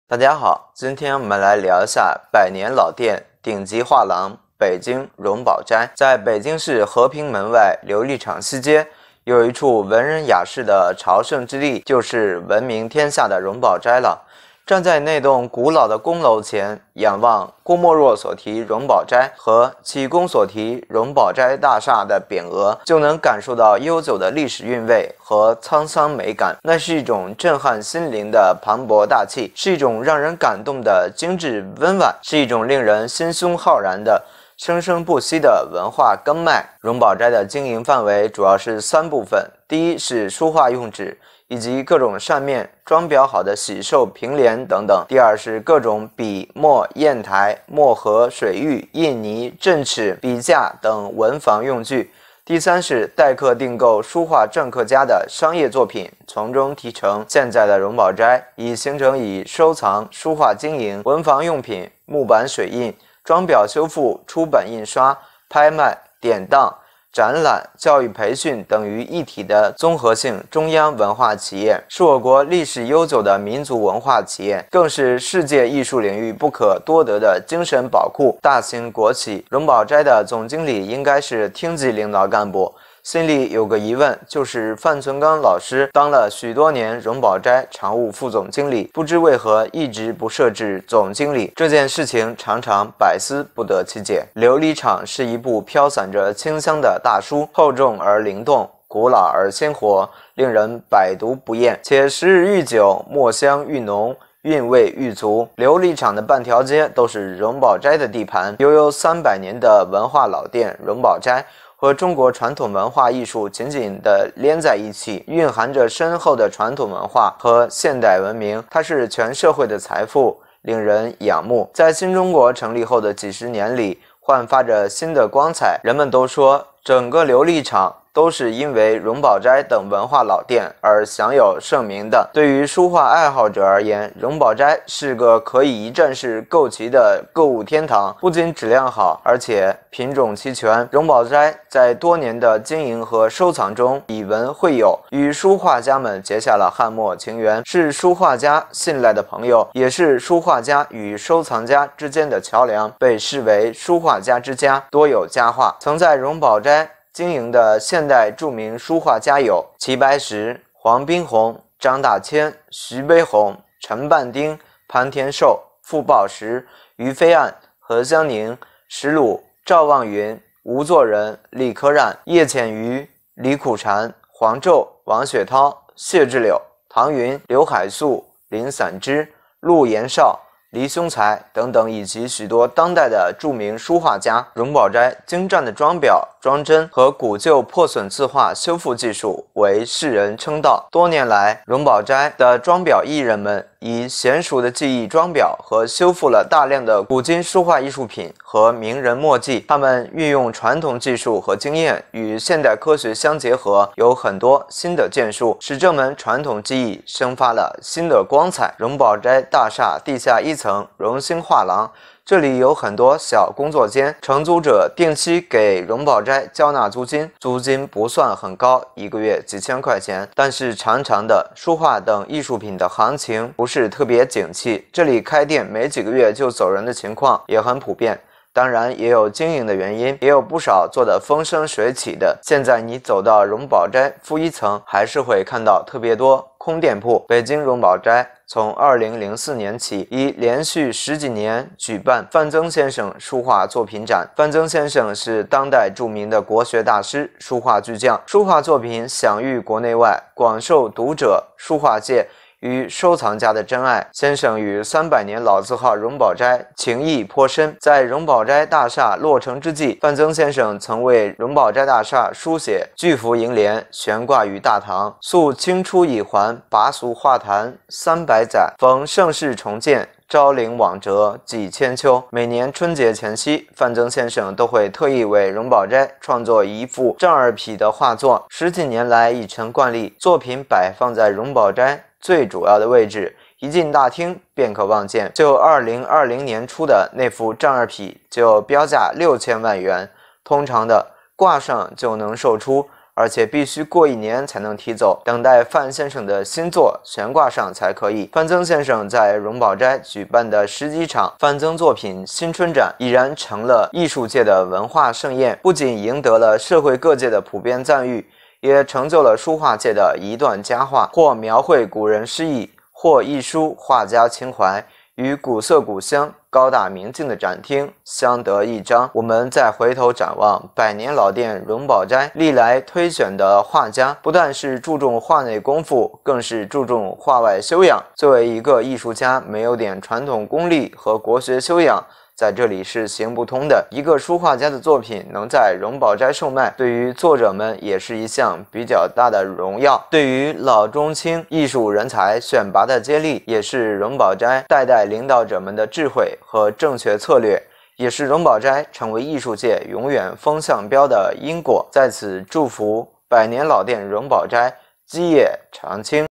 百年老店、顶级画廊，北京“荣宝斋”，艺术界永远的风向标|木上山房013期
北京荣宝斋从2004年起已连续十几年举办范曾先生书画作品展。范曾先生是当代著名的国学大师，书画巨匠，书画作品享誉国内外，广受读者、书画界与收藏家的珍爱。先生与三百年老字号荣宝斋情谊颇深，在荣宝斋大厦落成之际，范曾先生曾为荣宝斋大厦书写巨幅楹联悬挂于大堂：“溯清初以还 拔俗画坛三百载；逢盛世重建 昭灵往哲几千秋。”每年春节前夕，范曾先生都会特意为荣宝斋创作一幅丈二匹画作，十几年来已成惯例。作品摆放在荣宝斋最主要的位置，一进大厅便可望见。就2020年初的那幅丈二匹，就标价6000万元，通常的，挂上就能售出，而且必须过一年才能提走，等待范先生的新作悬挂上才可以。范曾先生在荣宝斋举办的十几场“范曾作品新春展”已然成了艺术界的文化盛宴，不仅赢得了社会各界的普遍赞誉，也成就了书画界的一段佳话。描绘古人诗意，或一抒画家情怀，与古香古色、高大明净的展厅相得益彰。